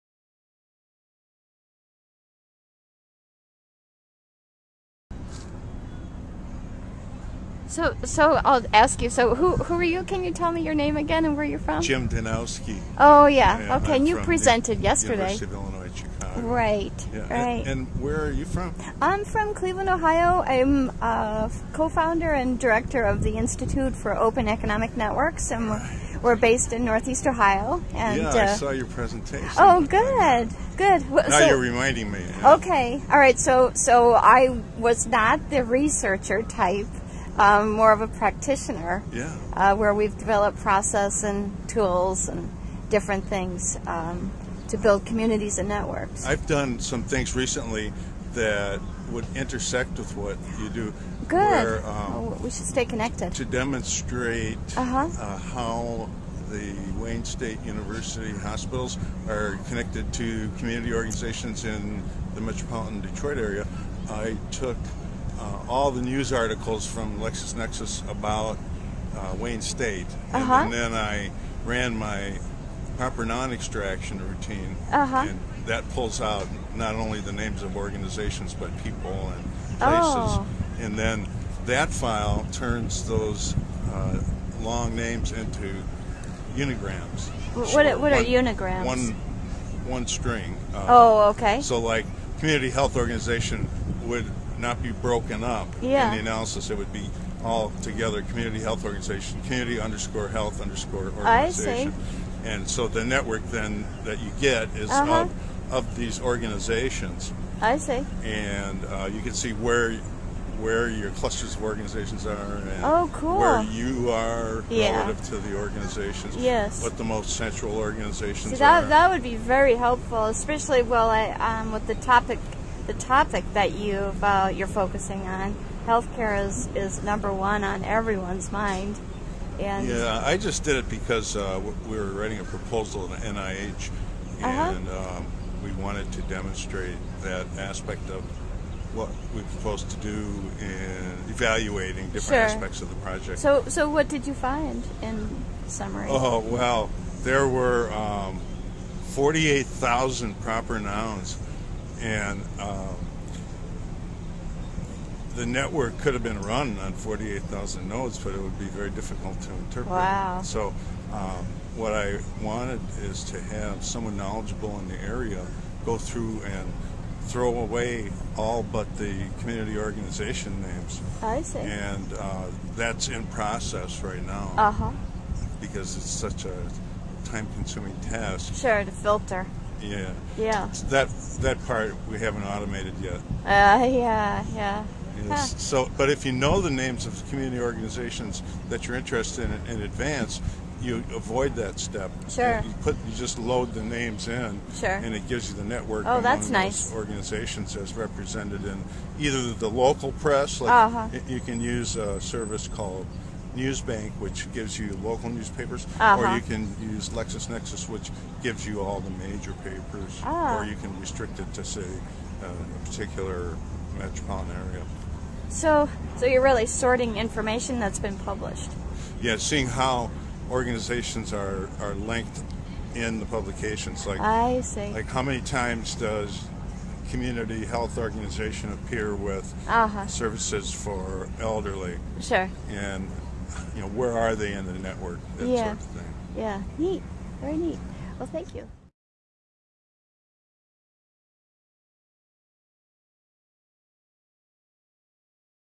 Conversations in Collaboration: Health Care
Jim Danowski PhD University of Illinois at Chicago talks about his work to understand how research universities connect to community organizations with a focus on health care. Jim cites one of his projects looking into the nature of connectivity between Wayne State University Hospitals and community organizations in the metropolitan Detroit MI region searching public news sources. Jim explains the strategy for collecting and filtering data and the story the resulting network mapping information tells.

You can download Jim's paper: Identifying an Organization's Interdepartmental Collaborative Innovation Networks from Mining Public Documents here:

Jim will be contributing to COINs 2010. Be sure to connect with Jim to learn more about his work in network mapping and analysis!

This interview was contributed by the Institute for Open Economic Networks (I-Open) in 2009.